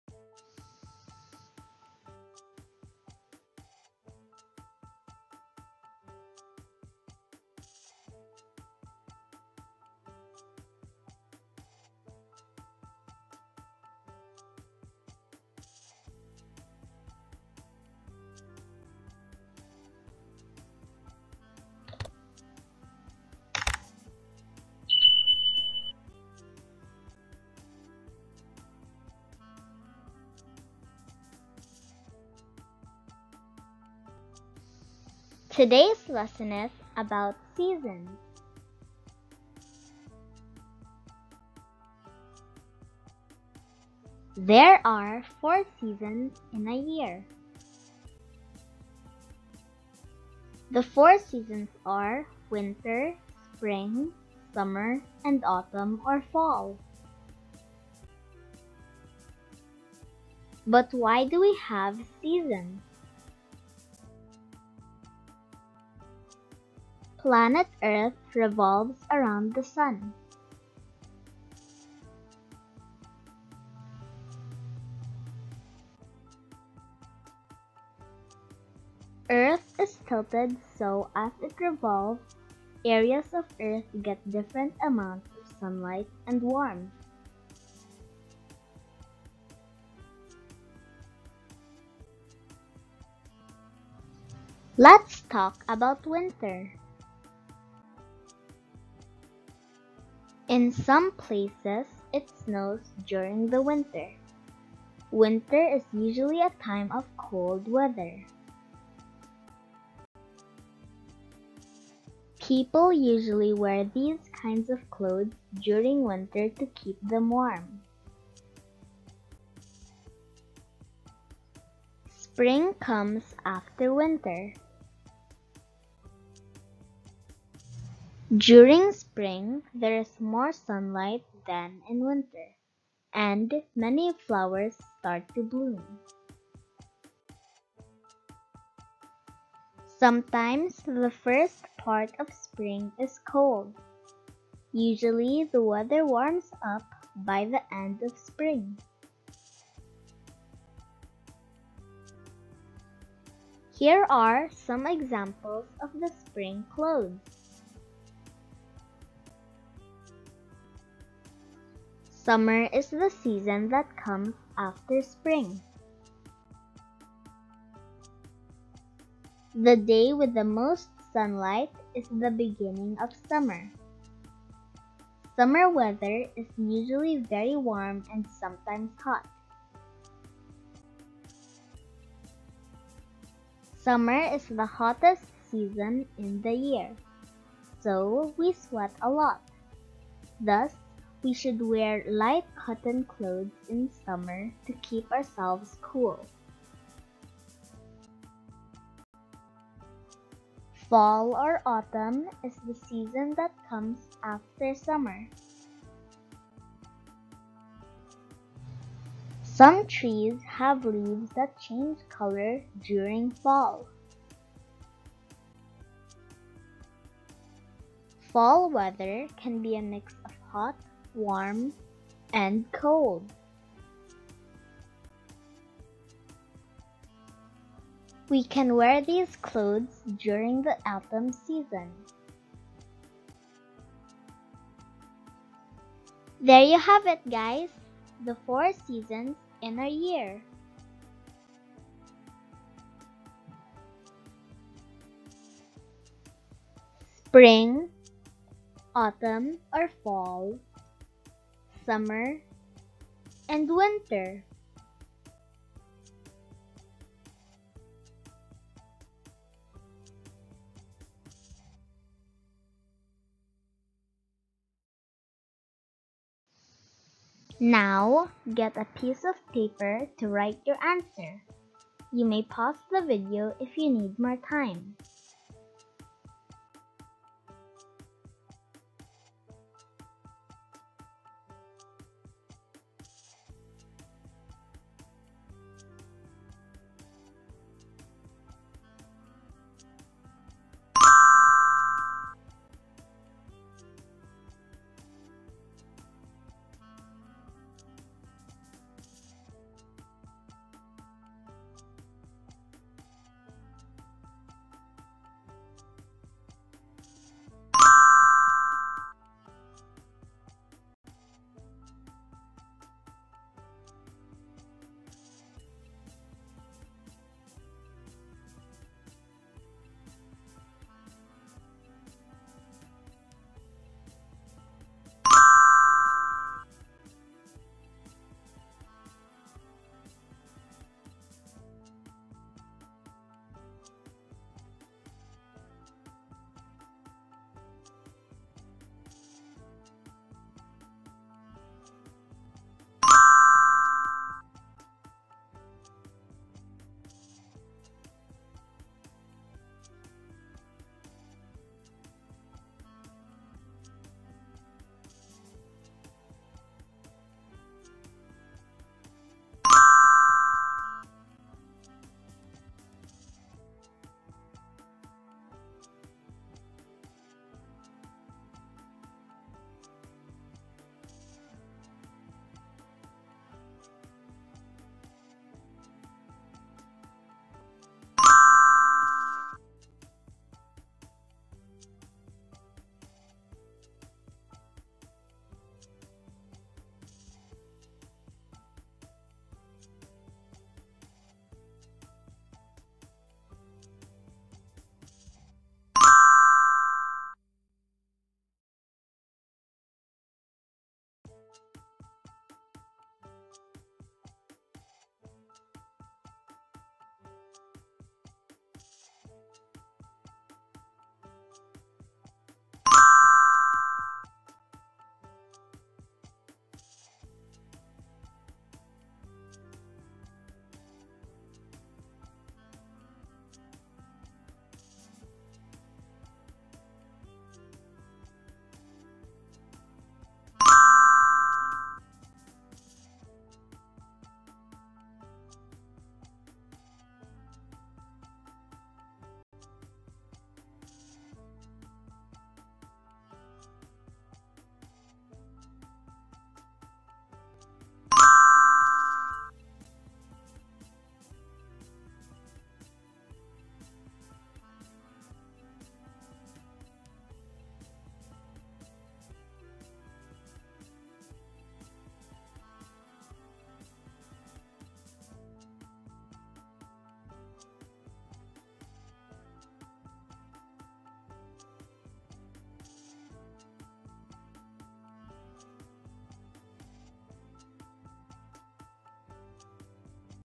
Seasons || Winter, Spring, Summer, Fall || Four Seasons || Liy Learns Tutorial
In temperate and sub-polar regions, four seasons based on the Gregorian calendar are generally recognized: spring, summer, autumn (or fall), and winter.